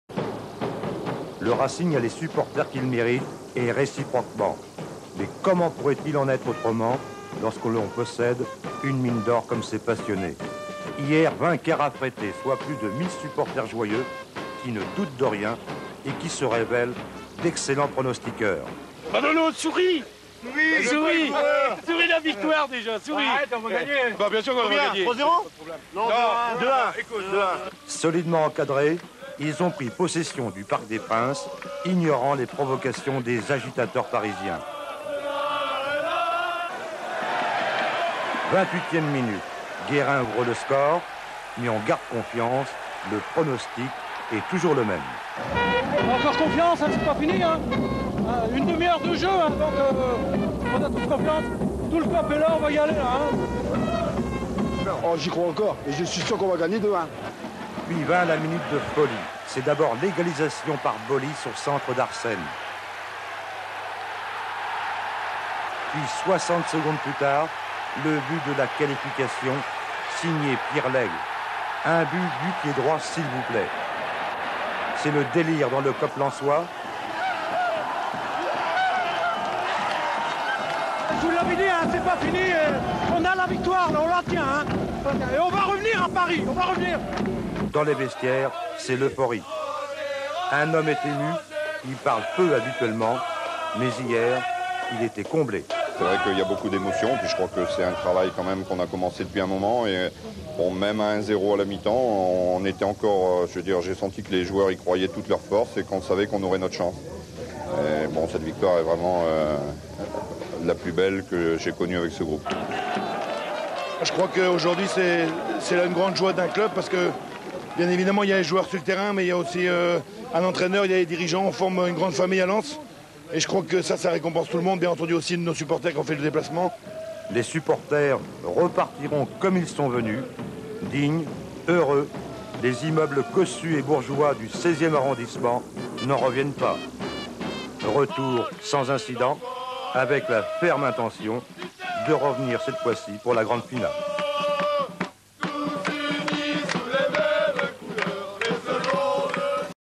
[Résumé] 23/04/1994 - Coupe de France - Quart de finale - PARIS SG LENS 1 - 2
Paris SG - Lens Première victoire au parc des princes pour le RC Lens !

Buts Lensois :
- Roger Boli (70)
- Pierre Laigle (72)

Buts Parisiens :
- Vincent Guérin (25)